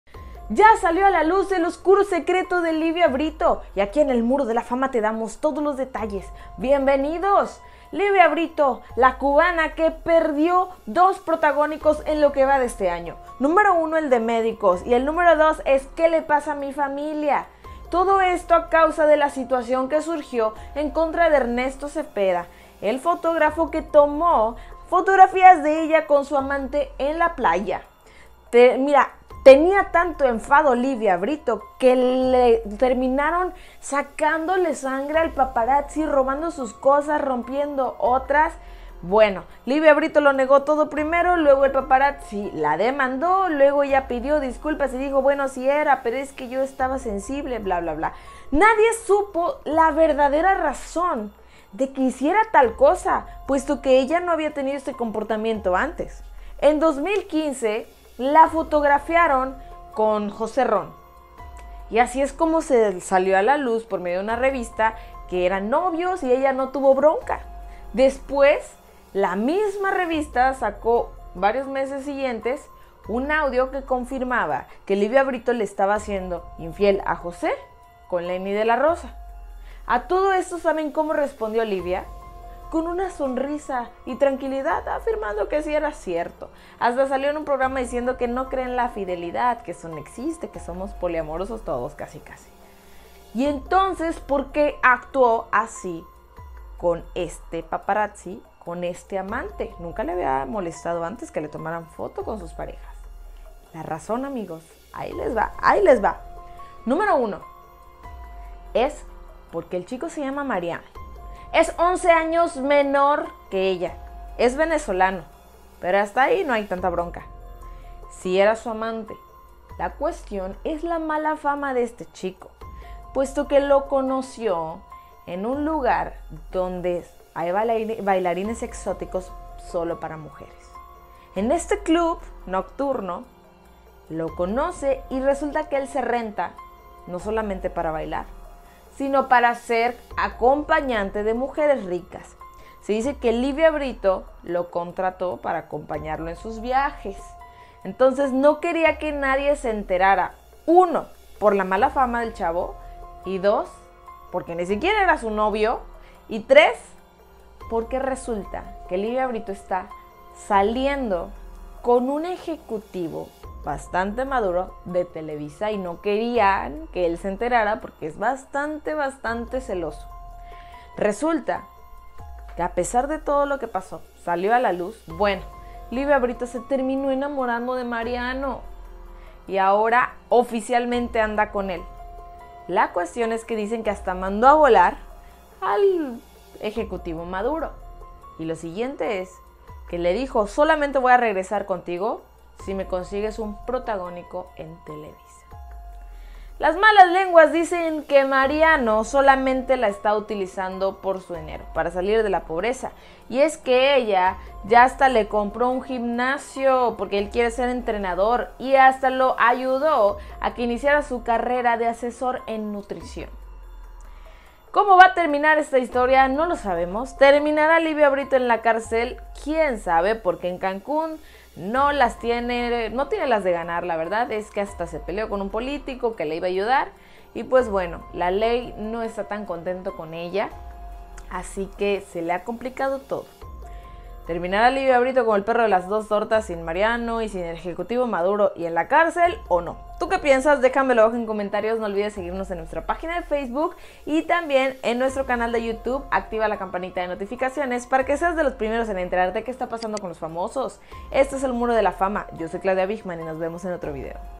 🔴🤐 ASI FUE 𝗖𝗼𝗺𝗼 𝗟𝗲 𝗗𝗲𝘀𝗰𝘂𝗯𝗿𝗶𝗲𝗿𝗼𝗻 El oscuro Secreto A 𝗟𝗶𝘃𝗶𝗮 𝗕𝗿𝗶𝘁𝗼. 🔴🤐
Este es el secreto guardado de livia brito y su amante

⭕🔕 𝗟𝗮 𝗘𝗰𝗵𝗮𝗻 𝗗𝗲 𝗧𝗲𝗹𝗲𝘃𝗶𝘀𝗮 A 𝗜𝘁𝗮𝘁í 𝗖𝗮𝗻𝘁𝗼𝗿𝗮𝗹 Por Esta Razon. Pero NO LO ESPERABA ⭕🔕

💗😲𝗔𝗻𝗮 𝗕𝗿𝗲𝗻𝗱𝗮 𝗖𝗼𝗻𝘁𝗿𝗲𝗿𝗮𝘀 REVELA El Nuevo Romance. Es Un Viudo 💗😲